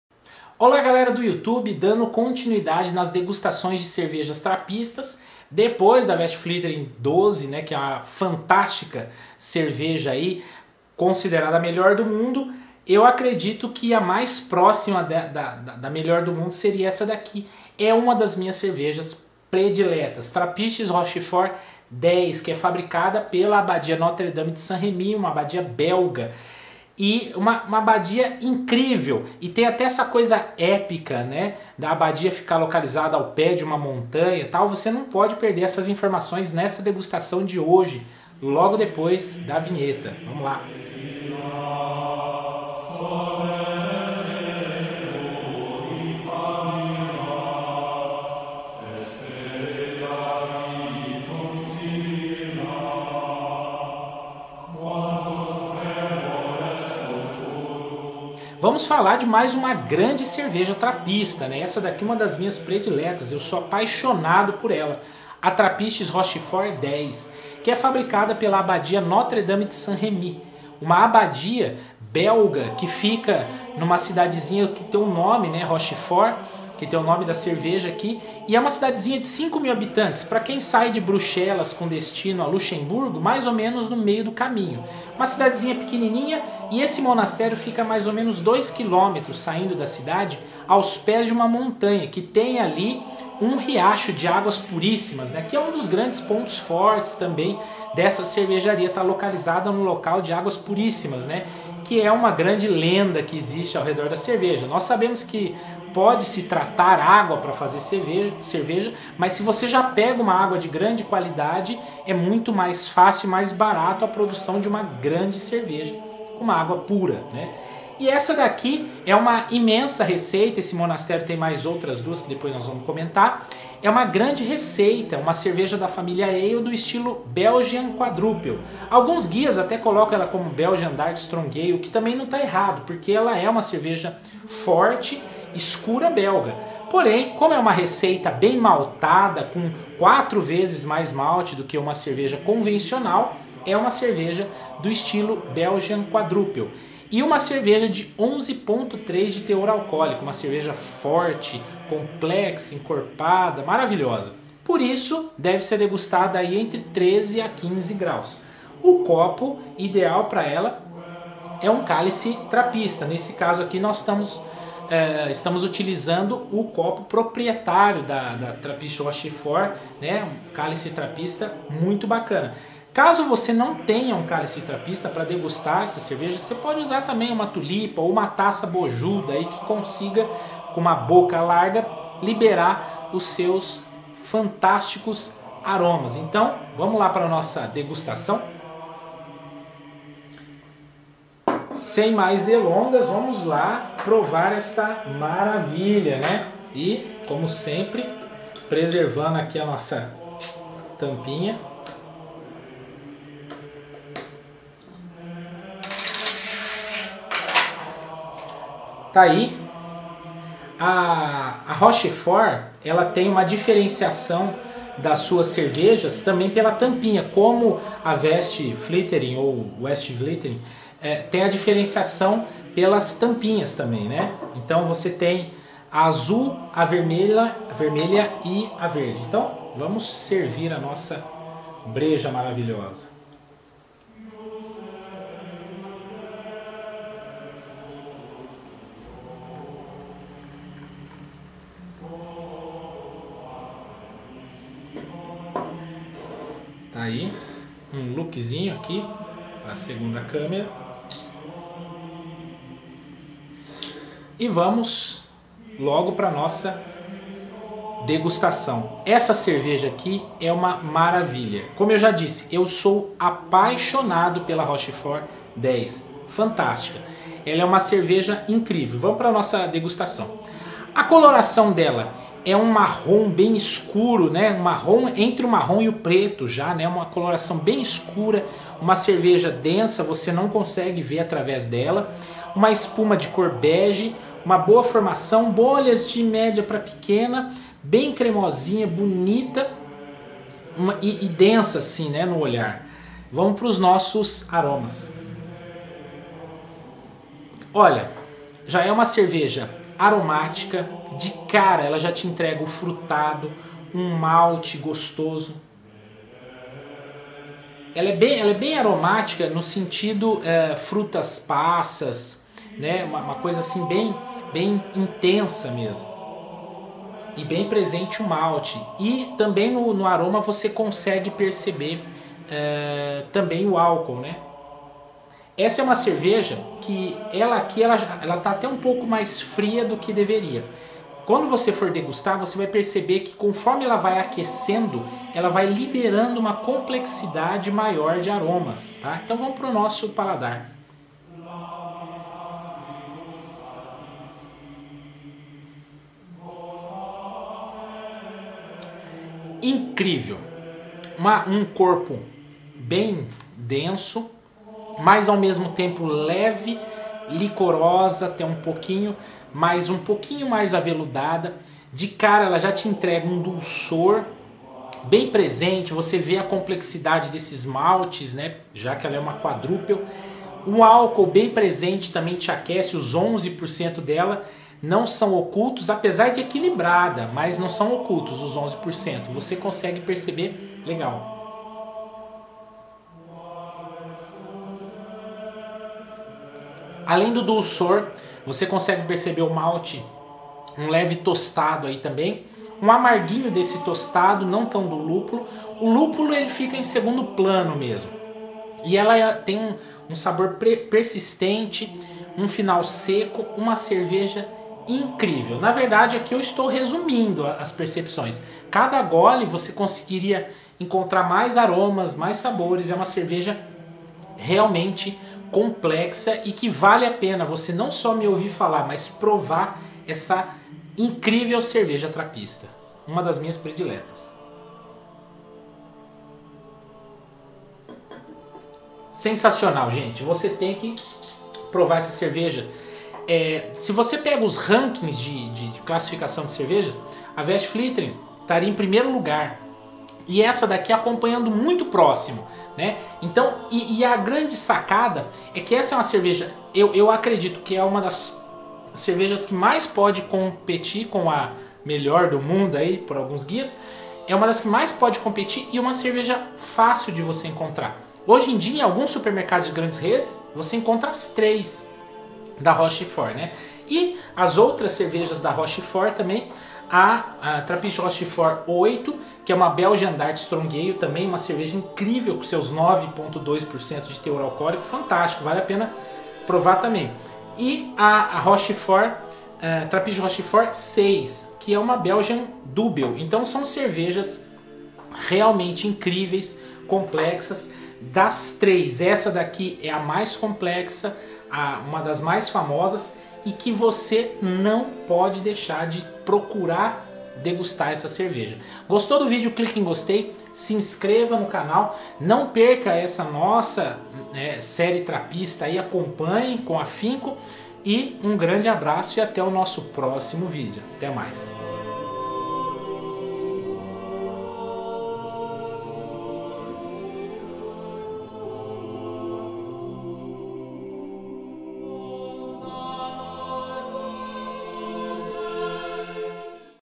Trappistes Rochefort 10 ResterBIER 43
🔥🔥🔥 Aprenda de forma prática e saia editando hoje mesmo! Aulão de DaVinci Resolve, em promoção, disponível no 👈 Confira agora mesmo! 🔥🔥🔥 Mais uma degustação de cerveja trapista, desta vez a maravilhosa Trappistes Rochefort 10, receita da Abbaye Notre-Dame de Saint-Remy.

FICHA TÉCNICA:

TRAPPISTES ROCHEFORT 10

Cervejaria: Abbaye Notre-Dame de Saint-Remy
País de Origem: Bélgica (Rochefort)
Família: ALE
Estilo: Belgian Quadrupel / ABT
Graduação Alcoólica: 11.3% ABV
Temperatura ideal para consumo: 13-15 °C
Copo Ideal: Cálice Trapista

Outras cervejas da mesma abadia:
- Trappistes Rochefort 8
- Trappistes Rochefort 6

Áudio do fundo:
"Cantos Gregorianos" de domínio público.
Public domain Gregorian Chant music.